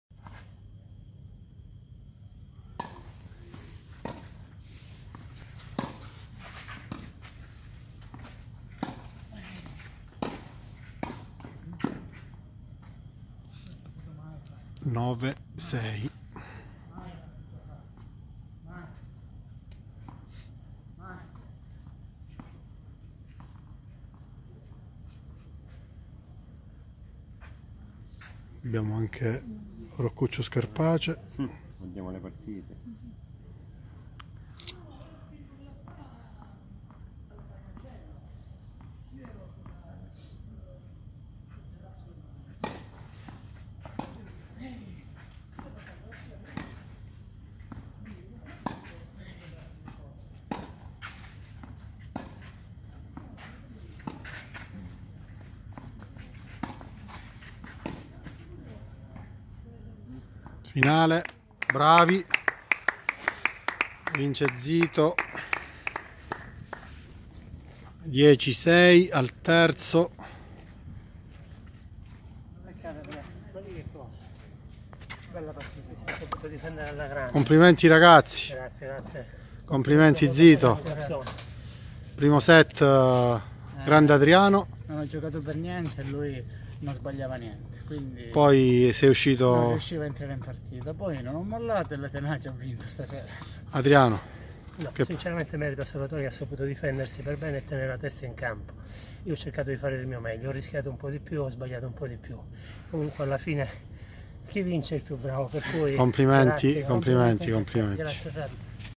Torneo di Tennis Gioiasport: fasi finali e commenti di Zito e Messina
Salvatore Zito e Adriano Messina impegnati nel tabellone Gold del IV Torneo Gioiasport